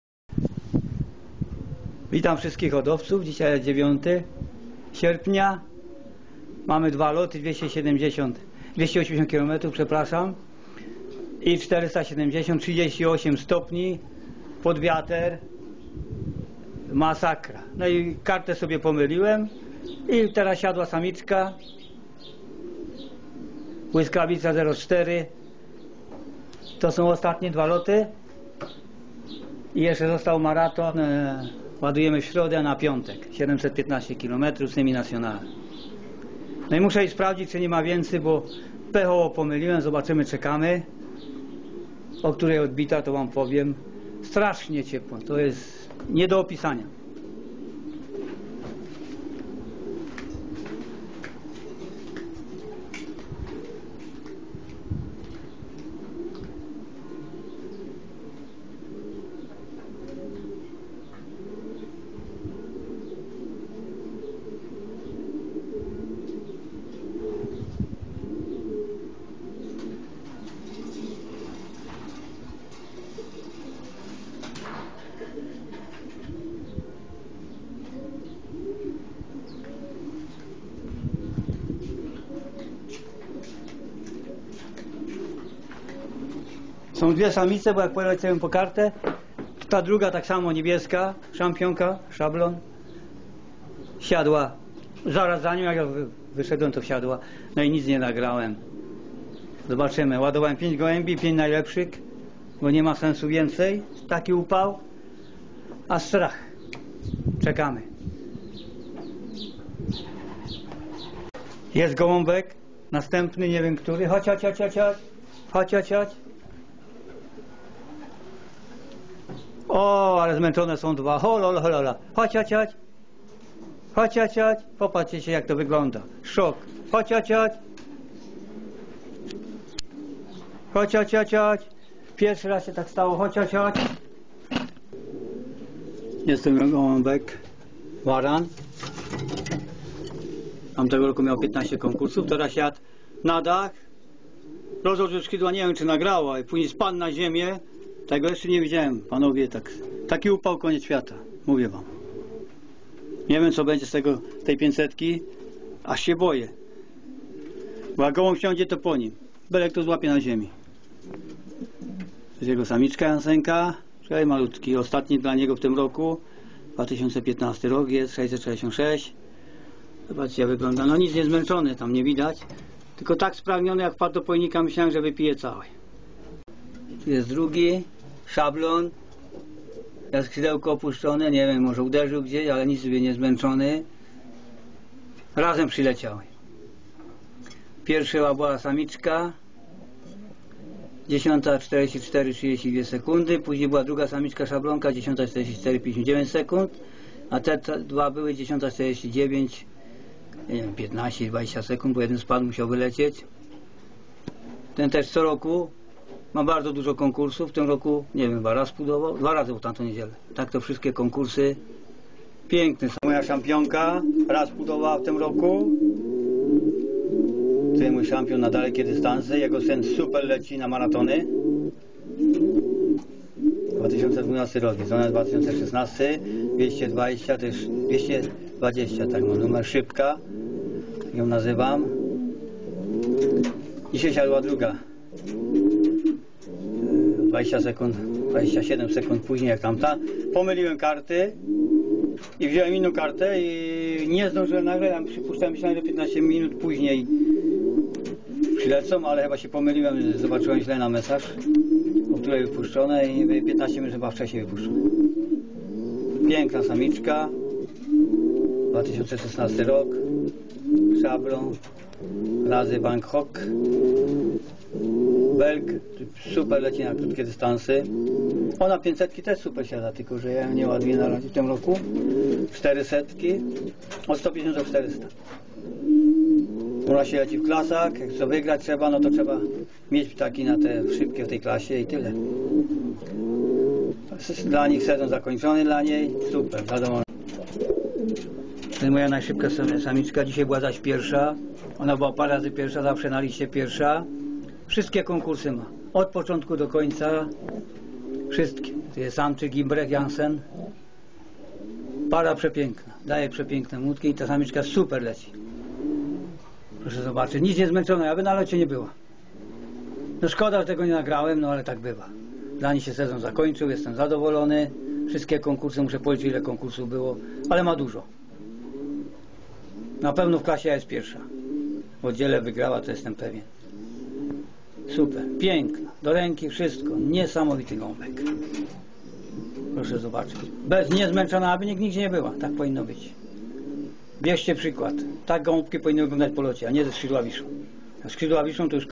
09.08.2020 ARGENTON - Piekielny Upał - 280 Km
Mistrzowska Hodowla Gołębi pocztowych "TURBO"

MIESZCZAK Józef

21 Region Paryjski (Francja)